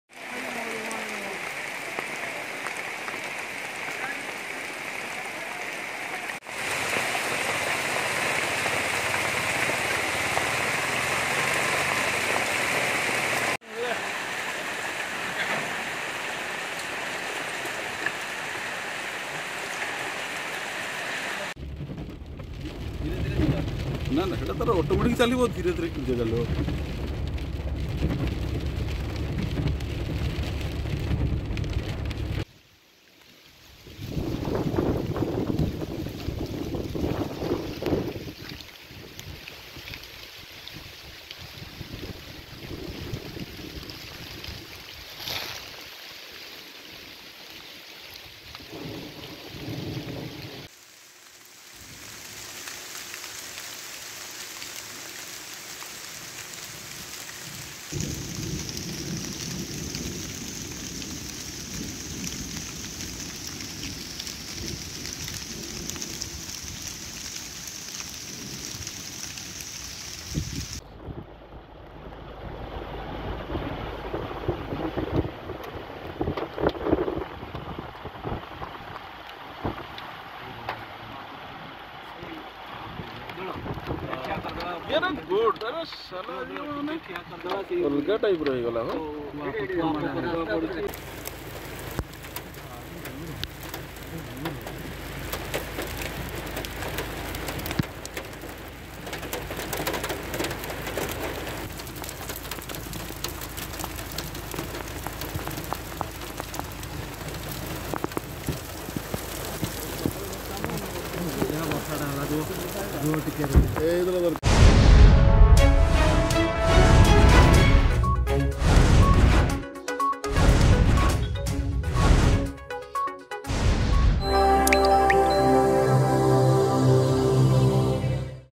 ମୟୁରଭଞ୍ଜ ଜିଲ୍ଲା କରଞ୍ଜିଆ ରେ ବର୍ଷା....
ମୟୁରଭଞ୍ଜ ଜିଲ୍ଲା କରଞ୍ଜିଆ ରେ ଦୀର୍ଘ 7 ମାସ ପରେ ବର୍ଷା
ବର୍ତ୍ତମାନ ତାପମାତ୍ରା ଦିନକୁ ଦିନ ବଢିଚାଲିଥିବା ବେଳେ ଅନ୍ୟପଟେ ରାଜ୍ୟର ବିଭିନ୍ନ ଜଙ୍ଗଲରେ ନିଆଁ ଲାଗିଛି। ଆଉ ଏତିକି ବେଳେ ପ୍ରକୃତିରାଣୀ ମଧ୍ୟ କୃପା ଦୃଷ୍ଟି ଦେଲେଣି.